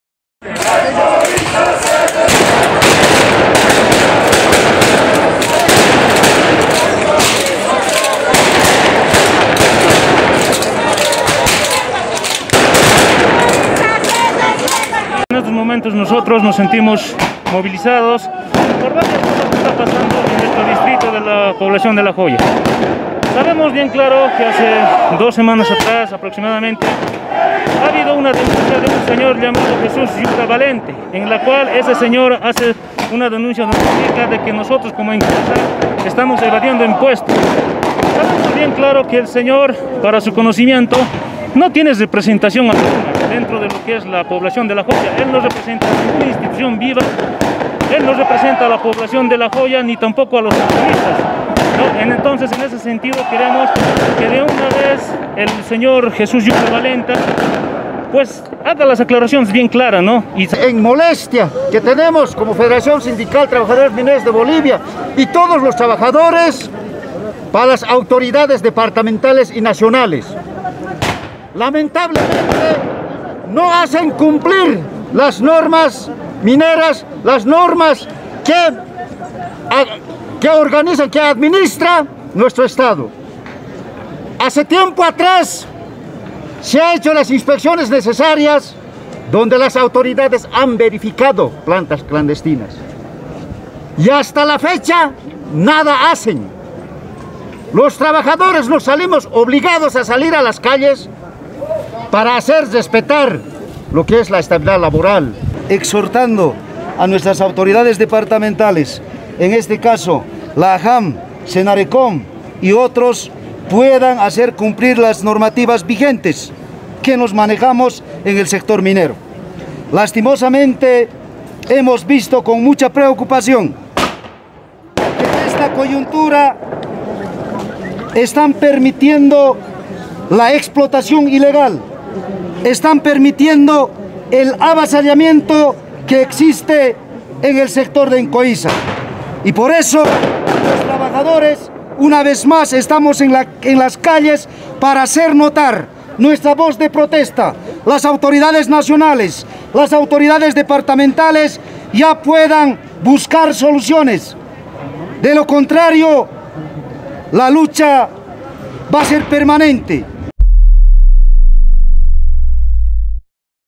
TRABAJADORES DE "EMCOISA" EN EMERGENCIA, DENUNCIAN EXPPLOTACIÓN ILEGAL
ADEMÁS DESMIENTEN ACUSACIONES DE POBLADORES DE LA JOYA